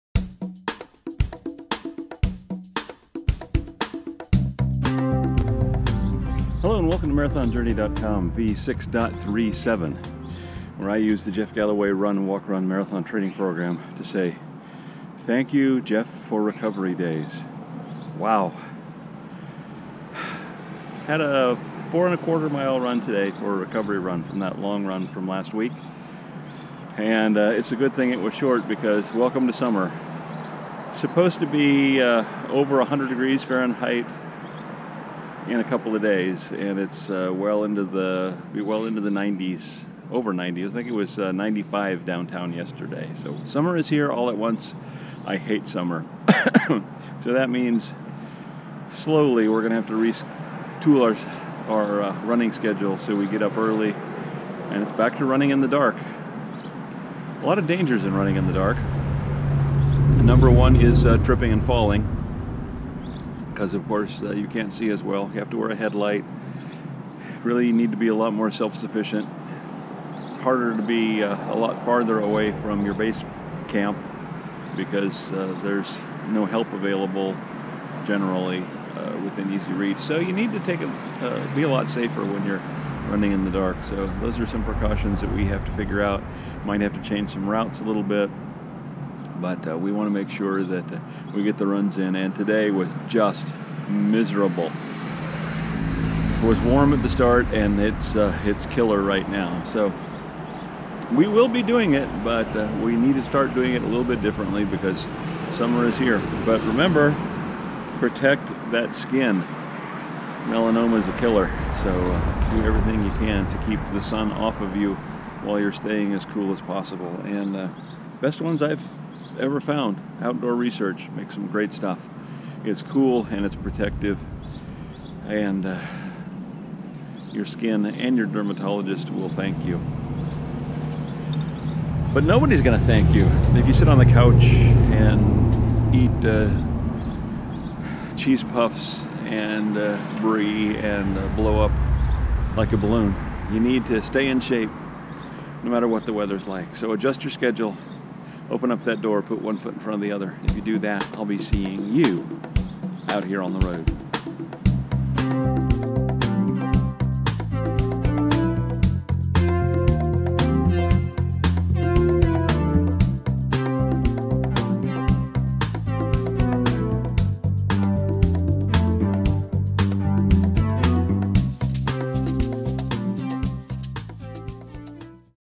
Enjoying a Recovery Run v6.37
A shorter run for a much hotter day.